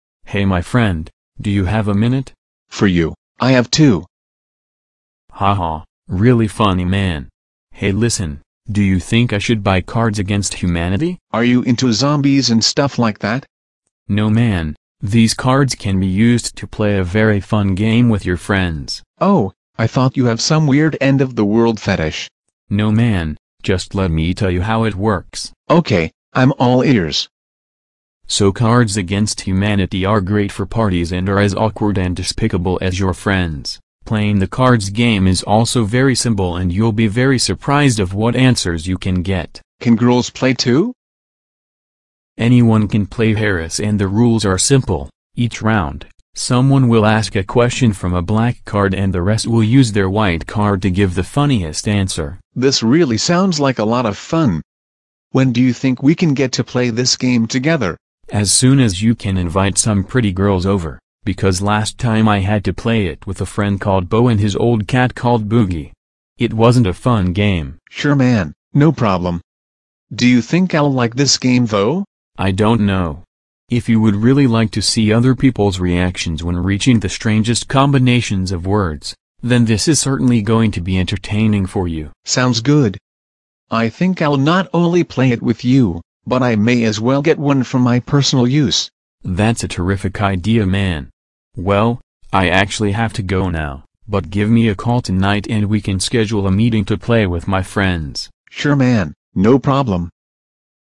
Should I Buy Cards Against Humanity?
Is the Cards Against Humanity game worth the money? Is it really as good as people say it is?

Harris my friend, do you have a minute?

For you Chris, I have 2.

Haha, really funny man. Hey listen, do you think I should buy cards against humanity?

Are you into zombies and stuff like that?

No man, these cards can be used to play a very fun game with your friends.

Oh, I thought you have some weird end-of-the-world fetish.

No man, just let me tell you who it works.

OK, I'm all ears.

So cards against humanity are great for parties and are as awkward and despicable as your friends. Playing the cards game is also very simple and you'll be very surprised of what answers you can get.

Can girls play too?

Anyone can play Harris and the rules are simple: each round, someone will ask a question from a black card and the rest will use their white card to give the funniest answer.

This really sounds like a lot of fun. When do you think we can get to play this game together?

As soon as you can invite some pretty girls over, because last time I had to play it with a friend called Bo and his old cat called Boogey. It wasn't a fun game.

Sure man, no problem. Do you think I'll like this game though?

Man I don't know. If you would really like to see other people's reactions when reaching the strangest combinations of words, then this is certainly going to be entertaining for you.

Sounds good. I think I'll not only play it with you, but I may as well get one for my personal use.

That's a terrific idea man. Well, I actually have to go now, but give me a call tonight and we can schedule a meeting to play with my friends.

Sure man, no problem!